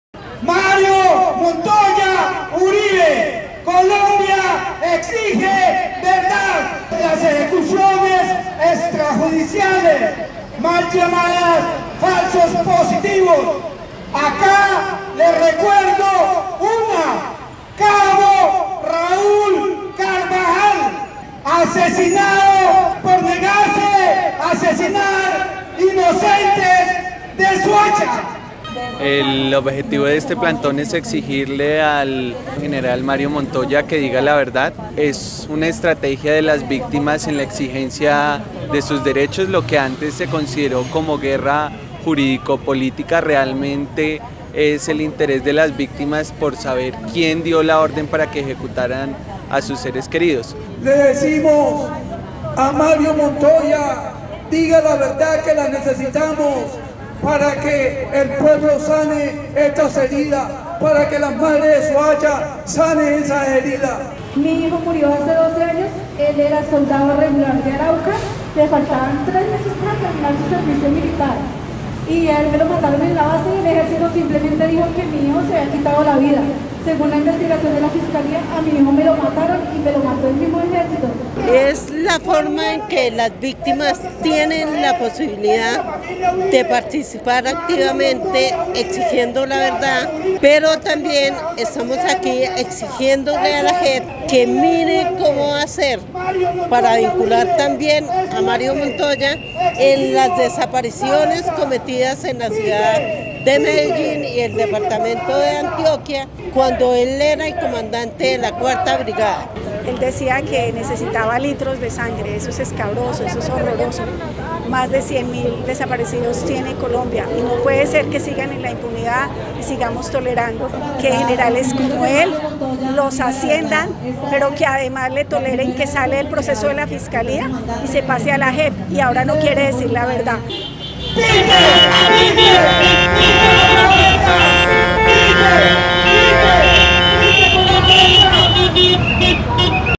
Plantón MontoyaDigaLaVerdad
El general en retiro Mario Montoya Uribe, comandante del Ejército Nacional entre febrero de 2006 y noviembre del 2008, empezó a rendir versión voluntaria el 12 de febrero dentro del trámite previsto por la Jurisdicción Especial para la Paz –JEP-. Fue convocado por la Sala de Reconocimiento de Verdad, de Responsabilidad y de Determinación de los hechos y Conductas, a rendir dicha versión en el marco del caso 003 “Muertes ilegítimamente presentadas como bajas en combate por agentes del Estado” durante los días 12, 13 y 14 de febrero de 2020. Han pasado casi cuatro años desde que la Fiscalía anunciara que le imputaría cargos por al menos 10 casos de los mal llamados “falsos positivos”, y año y medio desde que este general se acogiera a la JEP y firmara un compromiso de aporte a la verdad.

Las 209 versiones voluntarias rendidas hasta hoy en el caso 003 han permitido identificar la existencia de patrones como la alianza con paramilitares para cometer los crímenes, la utilización de informantes, la construcción de una narrativa común para negar los hechos, el uso, en varias unidades militares del llamado “kit” de legalización (usado para dar apariencia de combate a las ejecuciones extrajudiciales y así uniformar y armar a las víctimas asesinadas), la alteración de órdenes de batallas y otros documentos oficiales, así como irregularidades en pagos de recompensas, además de las acciones para encubrir dichas prácticas criminales.

Todos estos patrones demuestran que se trató de una práctica planificada y generalizada que se presentó en casi la totalidad de departamentos del país y de la cual no pudo estar ajena al máximo comandante del ejército nacional, así como tampoco las instancias militares superiores y el gobierno nacional.

Por eso para las víctimas y sus representantes no tiene ningún sentido que el General (r) Montoya Uribe se acoja a un mecanismo de justicia transicional sin ningún ánimo de contribuir a la verdad, que es máximo pilar del SIVJRNR para esclarecer y juzgar a los máximos responsables de los crímenes más graves y representativos cometidos en el contexto del conflicto armado.